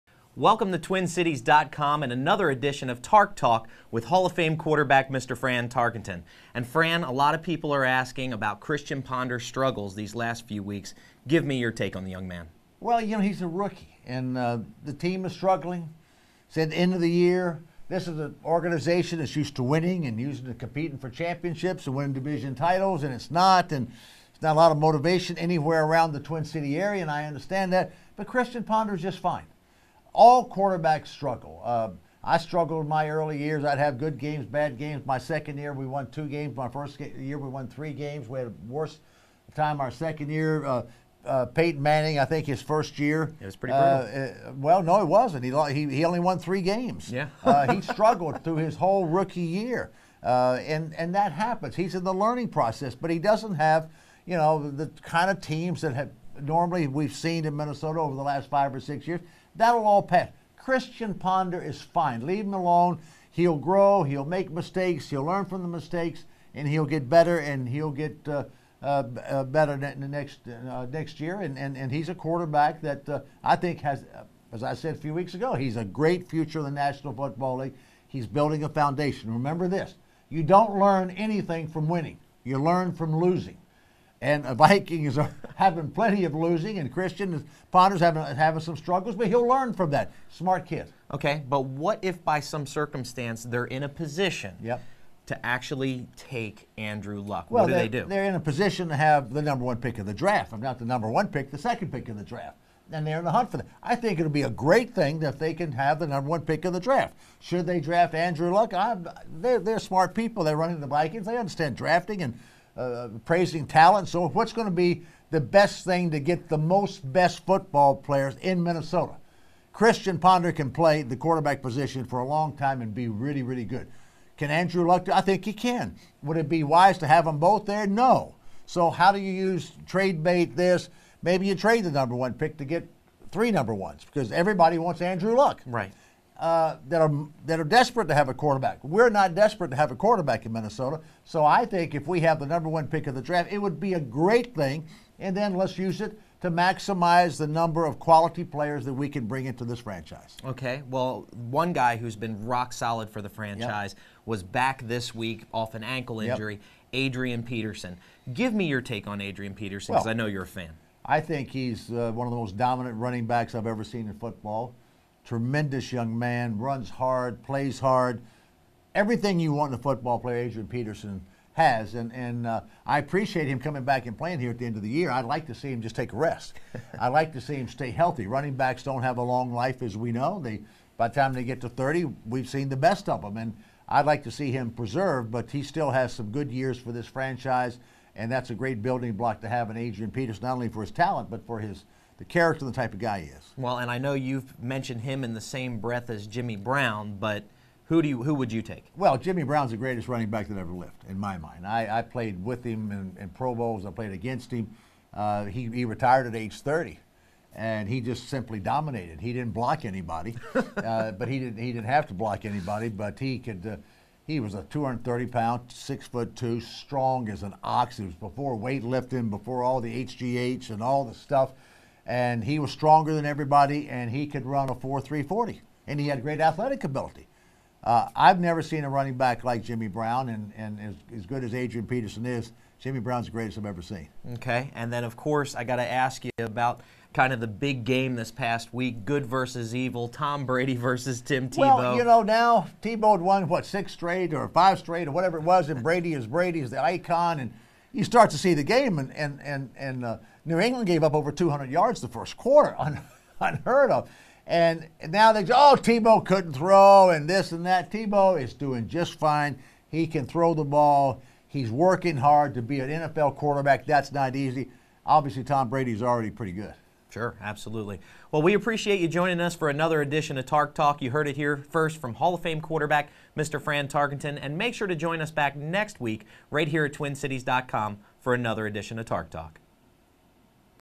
Former Vikings QB Fran Tarkenton: 'Christian Ponder is fine'
Frant Tarkenton says he struggled as a rookie quarterback, too -- and so did Peyton Manning. Christian Ponder is no different.